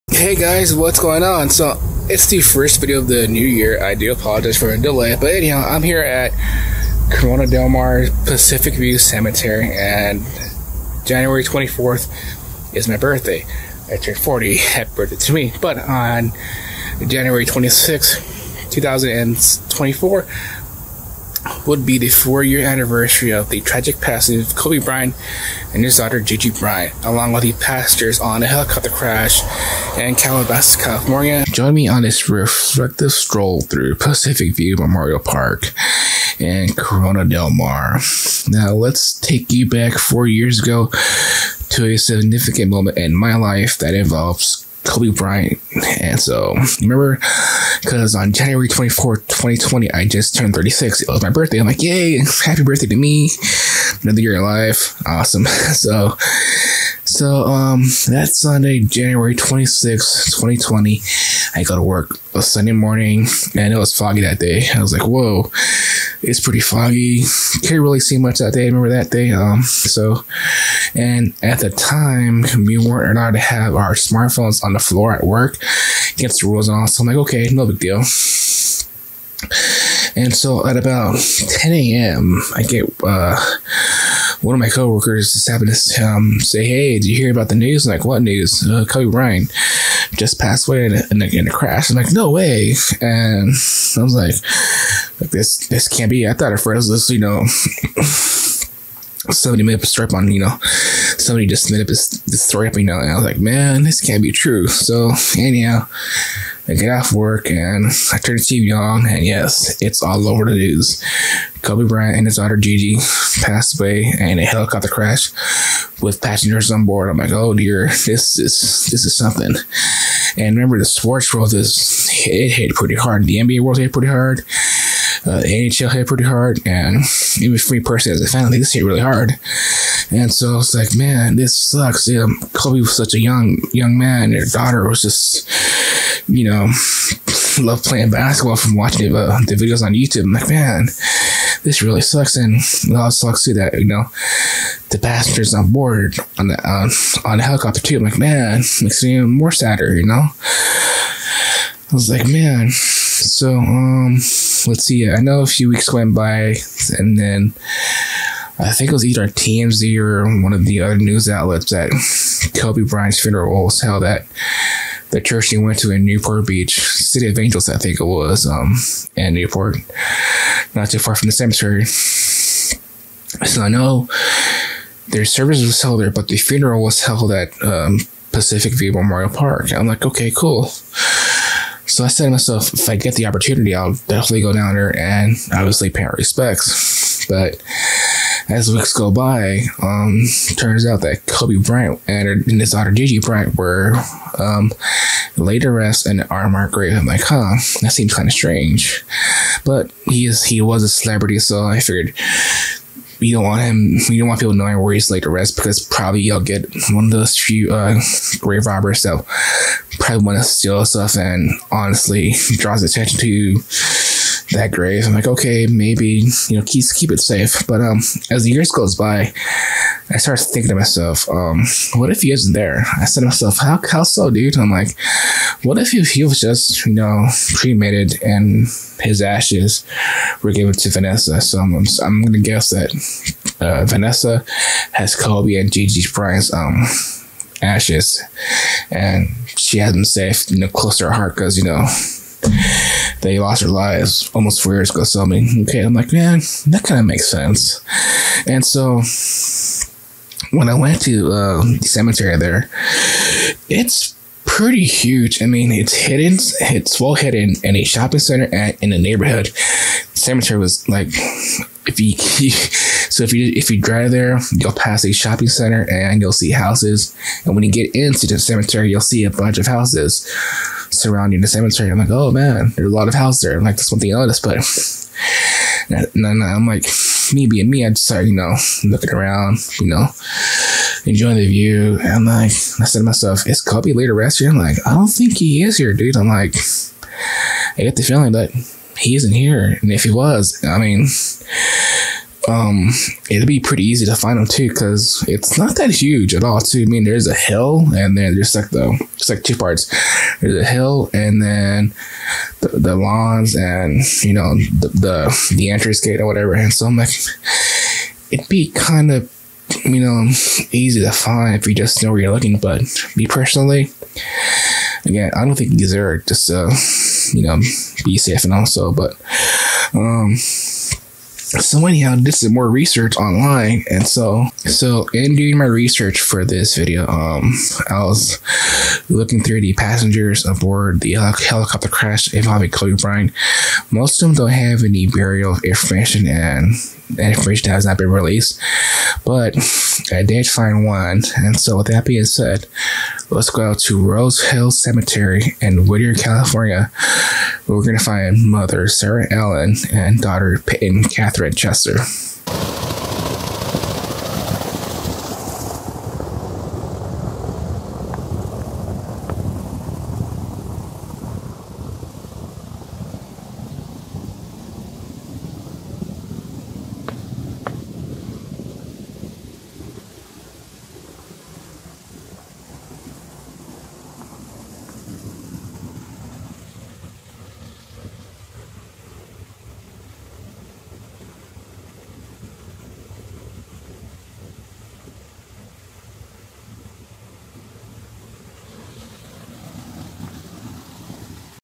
Reflecting on the 4-Year Anniversary of Kobe Bryant | A Visit to Pacific View Memorial Park
Join me in a heartfelt tribute to Kobe Bryant and the passengers as we remember the 4-year anniversary. This video takes you on a journey of reflection and remembrance, honoring the lives affected by the tragic events. Let's cherish the memories together.